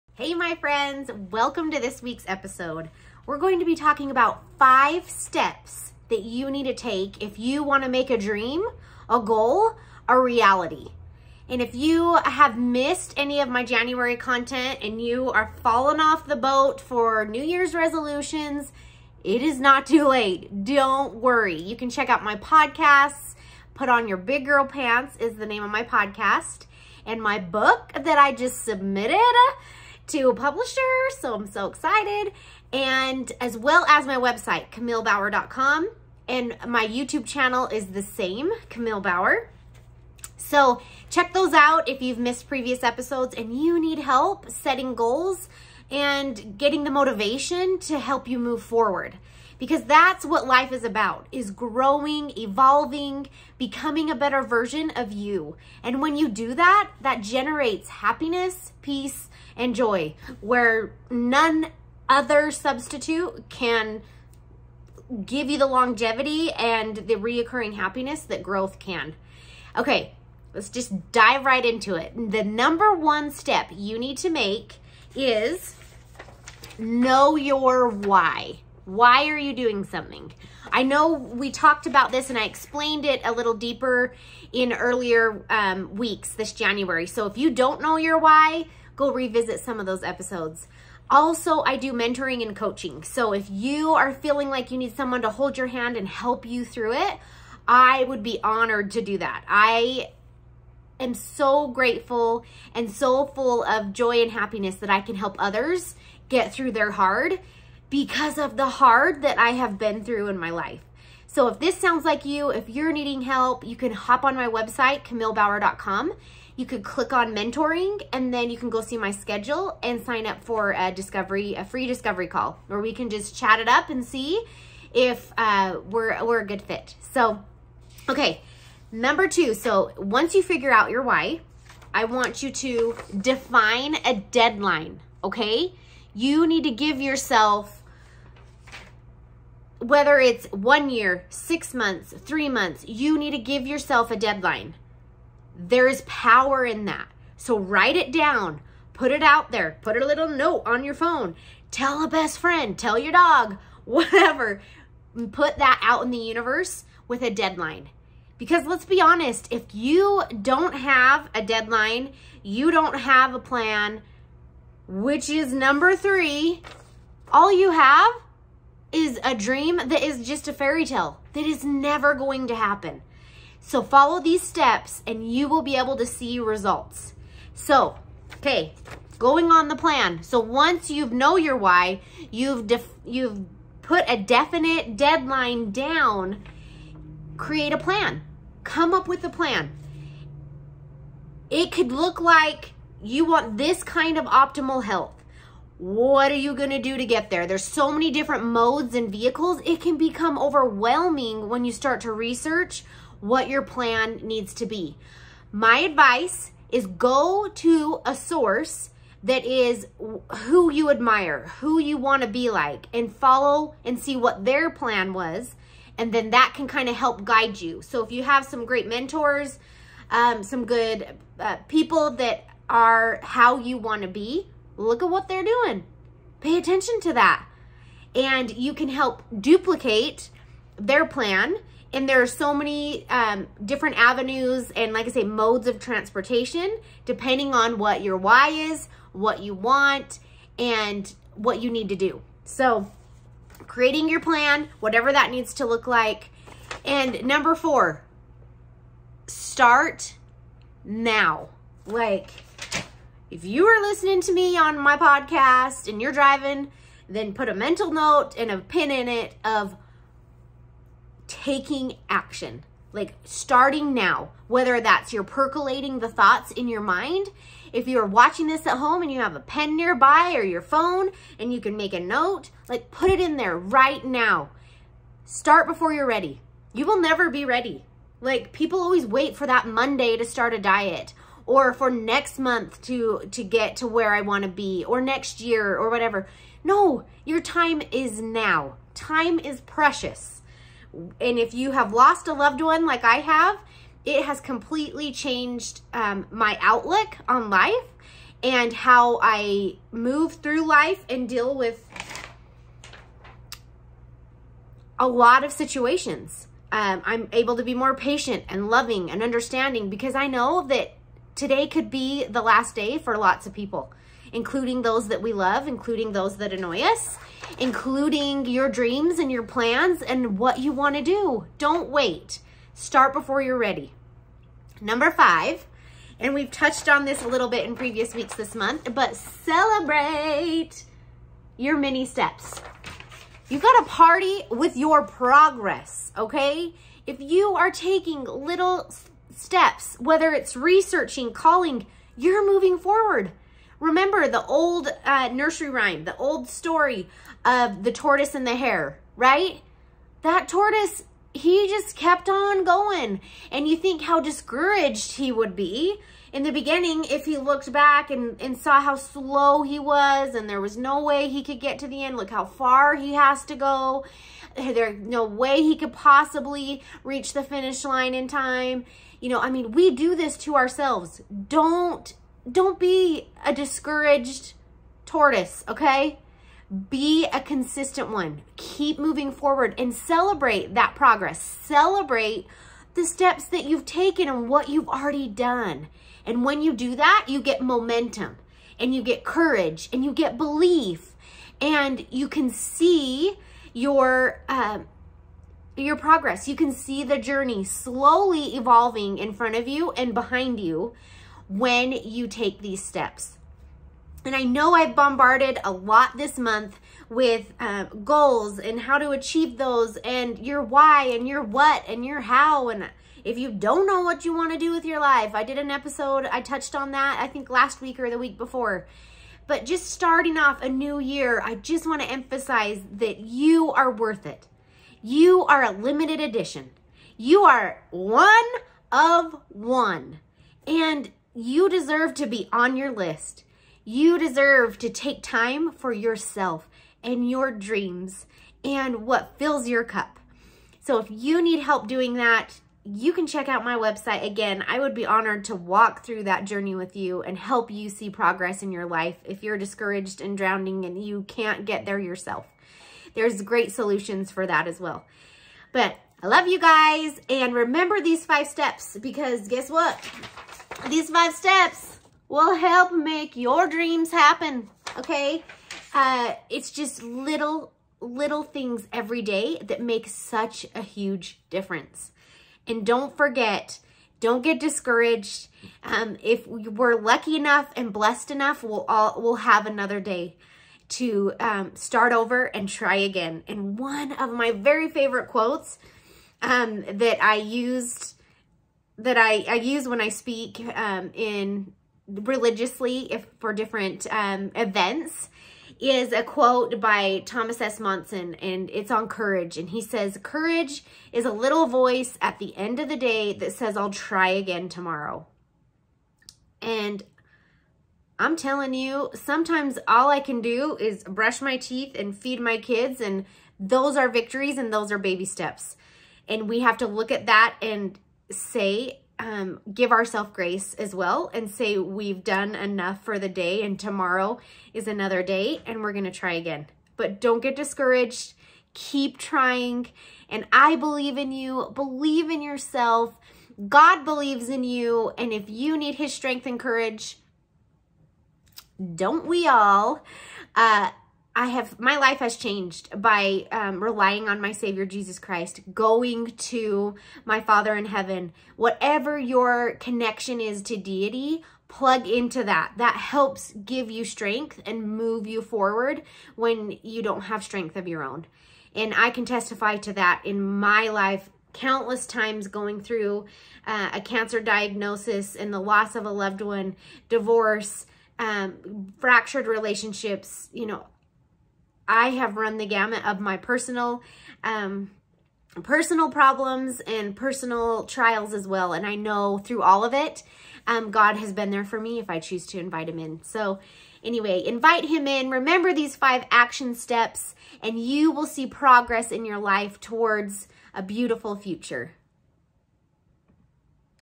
Make Your Dreams Come True | 5 Steps to Take
Are you struggling seeing any progress towards your goals and dreams? Do you need help making your dreams a reality? In this week's episode Kamille gives 5 crucial steps that will help you.

Don't give up if you are hurt. If you need help with your challenges you can checkout and schedule yourself a free 30 minute call to see if Kamille is a good fit for you to walk your journey into increased hope, peace, and happiness.